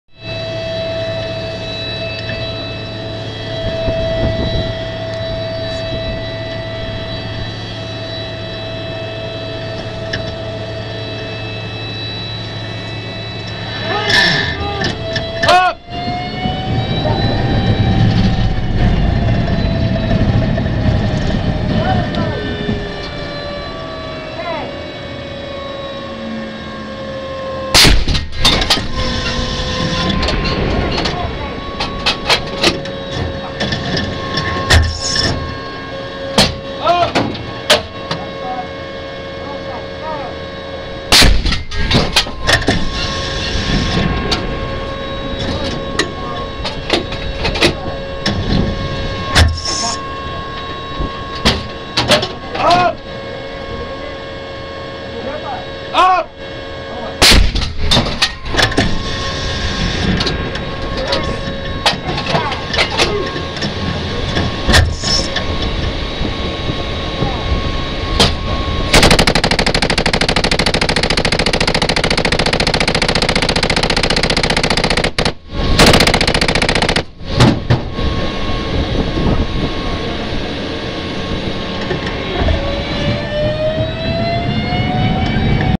Mad Minute FPS Style
The infamous "Mad Minute" engagement with 3 tanks and a set of troops versus 1 fully operational M1A1 MBT.